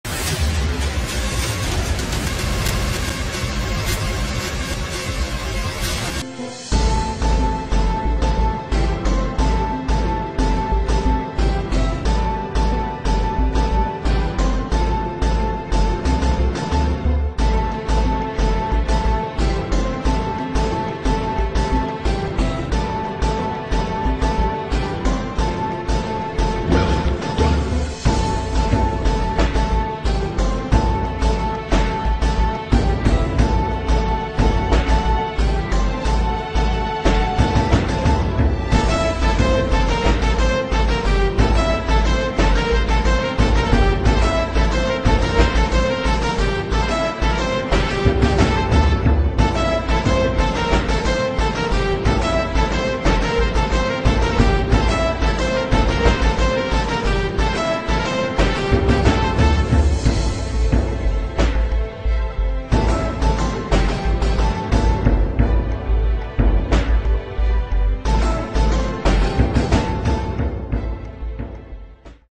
Mortal Kombat 2021 movie - Scorpion defreezing Speed Painting
Movie: Mortal Kombat 2021
Program: Krita